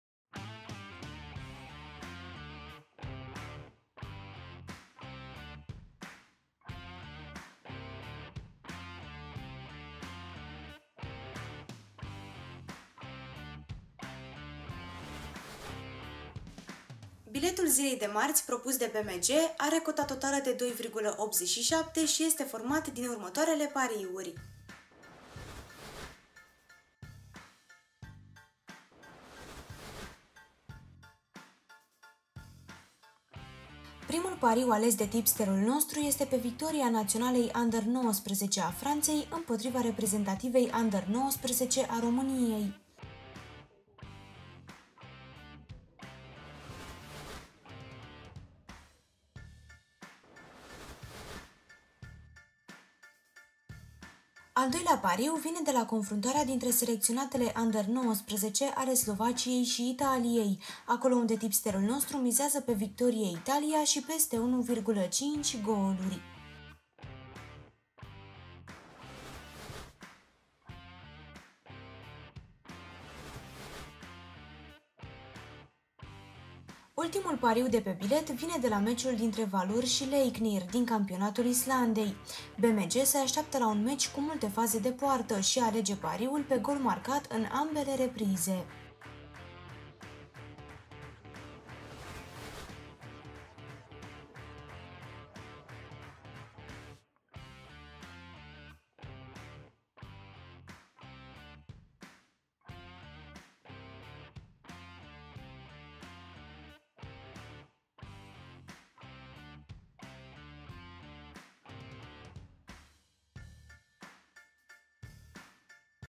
Biletul Zilei COTA 2 - 21.06.2022
Biletul zilei COTA 2 la pariuri pentru marti (21 iunie 2022), propus de Bmg, este format din trei pariuri si are cota totala de 2.87